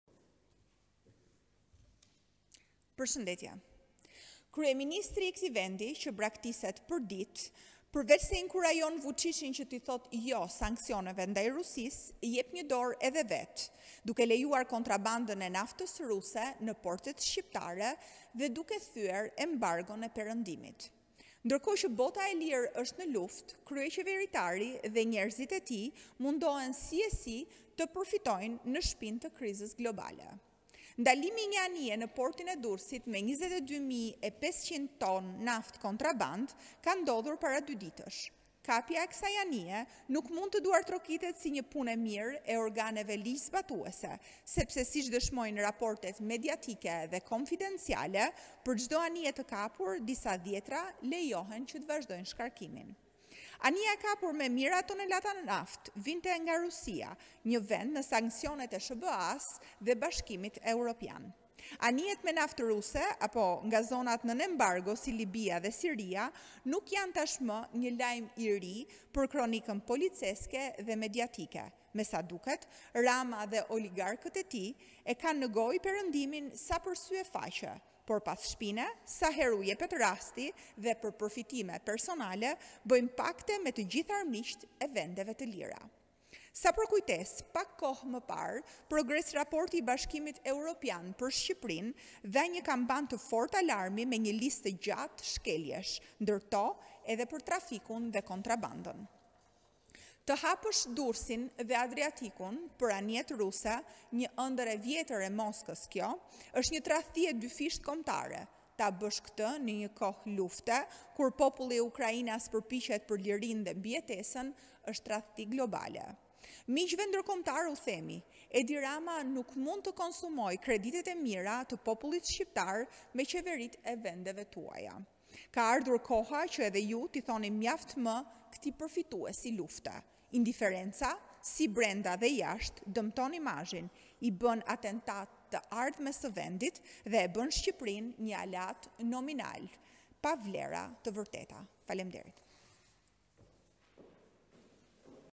Bllokimi anijes ruse me me 22.500 tonë naftë kontrabandë në Portin e Durrësit,Rithemelimi: Tradhëti
Rithemelimi ka akuzuar kryeministrin Edi Rama se ka lejuar kontrabandën e naftës ruse në portet shqiptare duke iu referuar rastit më të fundit të kapjes së anijes që transportonte 22.500 tonë naftë në portin e Durrësit.